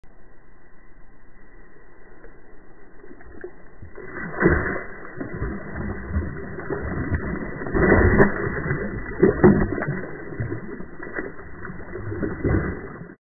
Monster Bass Leaps for Shad!
In this video, a giant bass from my pond leaps out of the water to grab a big shad dangling from the feeding rod.